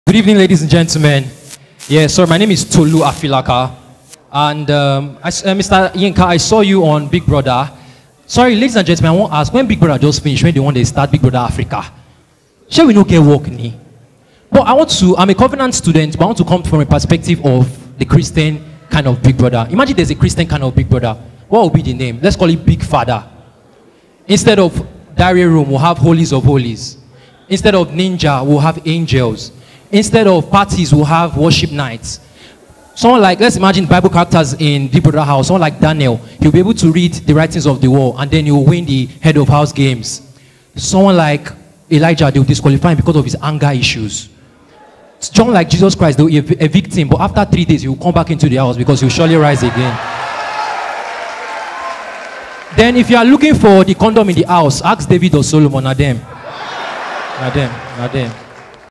THIS COMEDIAN CREATED CHRISTIAN BIG BROTHER | TOLU AFILAKA
Tolu Afilaka Cracks in less than 1 minute about having a Christian type of big brother,